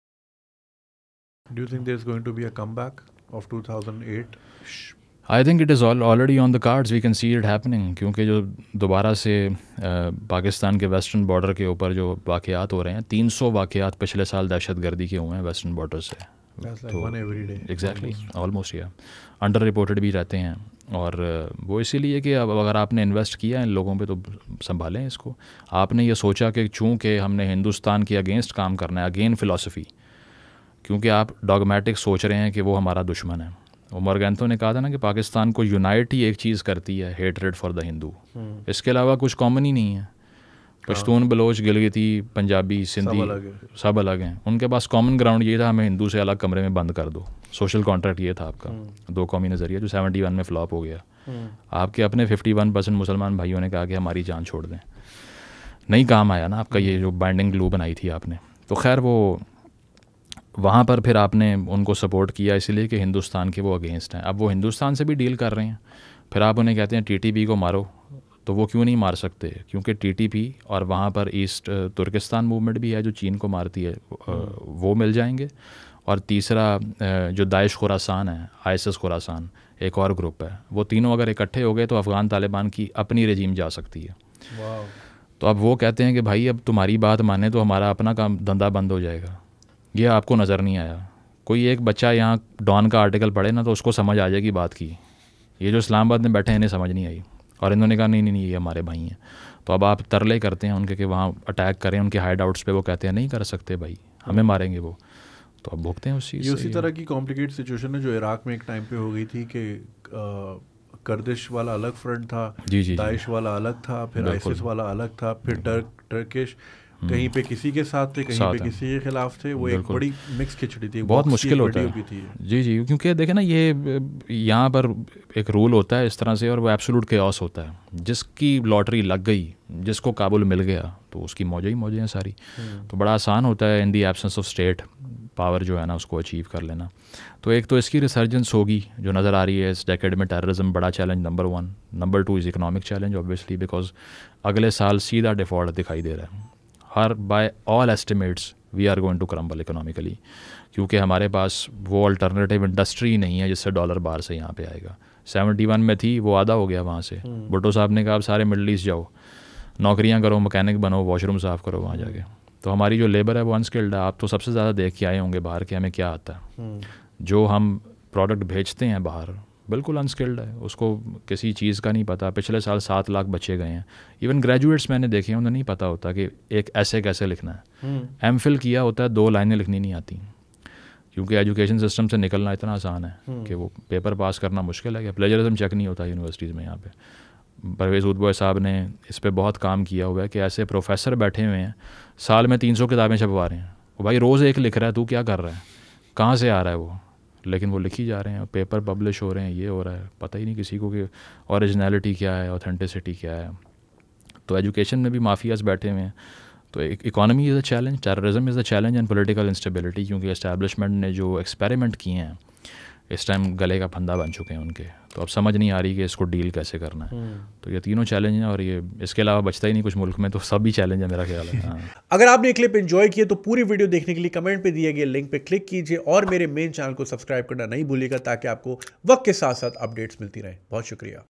The Rise Of Teror**m | Junaid Akram Clips
In conversation with Syed Muzammil Shah regarding the rise of terro**st Attacks on the borders. Escalating situations around the world.